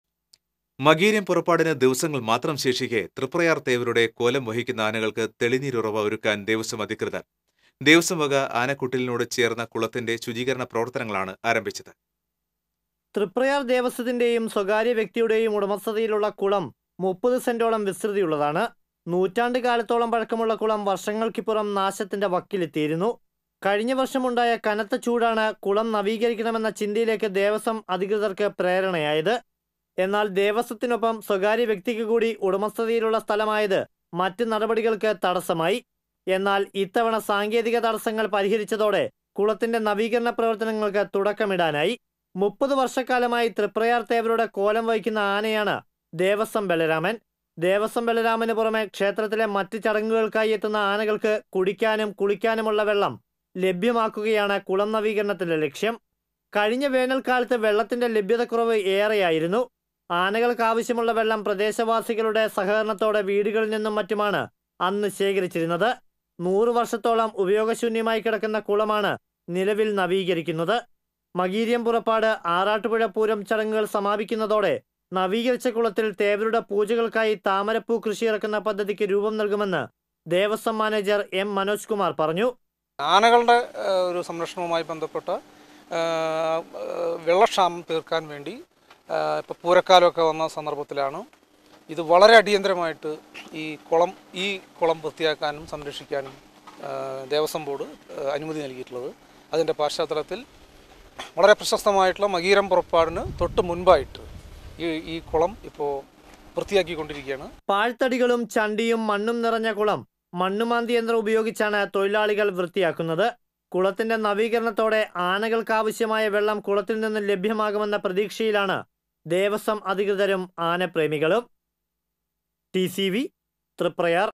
ആനകള്‍ക്ക് തെളിനീരുറവ ഒരുക്കാന്‍...| TCV Thriprayar
മകീര്യം പുറപ്പാടിന് ദിവസങ്ങള്‍ മാത്രം ശേഷിക്കെ തൃപ്രയാര്‍ തേവരുടെ കോലം വഹിക്കുന്ന ആനകള്‍ക്ക് തെളിനീരുറവ ഒരുക്കാന്‍ ദേവസ്വം അധികൃതര്‍. ദേവസ്വംവക ആനക്കൊട്ടിലിനോട് ചേര്‍ന്ന കുളത്തിന്റെ ശുചീകരണ പ്രവര്‍ത്തനങ്ങളാണ് ആരംഭിച്ചത്.